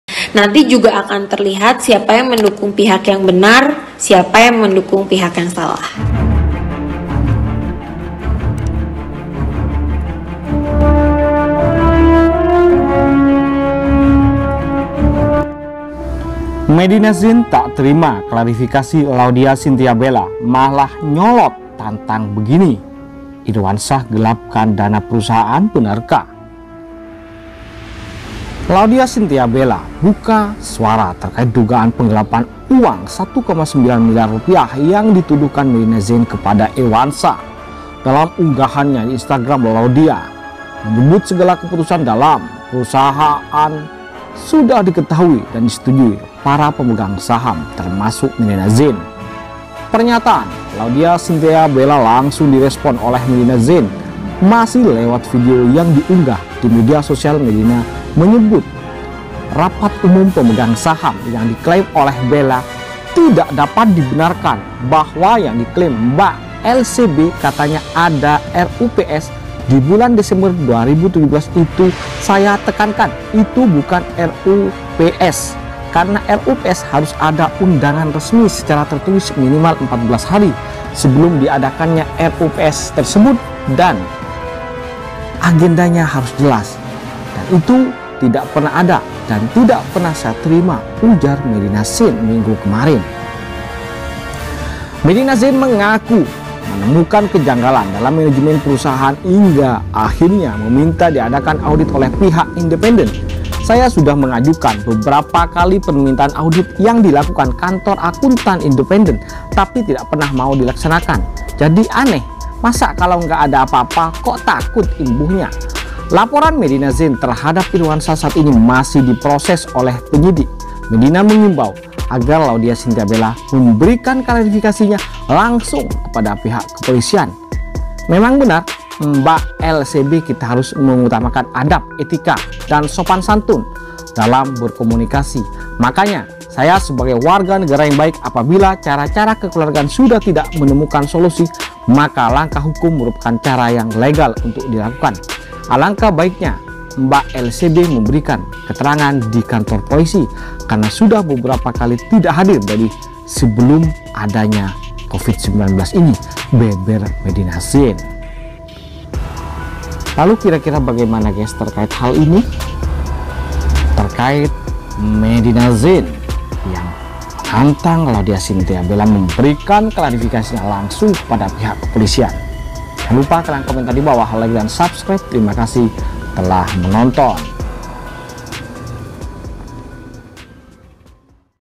Medina Zein Tak Terima Klarifikasi Laudya Cynthia Bella,Malah NGOTOOT Tantaang Begini?
Medina Zein Tak Terima Klarifikasi Laudya Cynthia Bella,Malah NGOTOOT Tantaang Begini,Irwansyah Gelaapkan Uang Perusahaan?

Jangan Lupa: _
Thankz!!..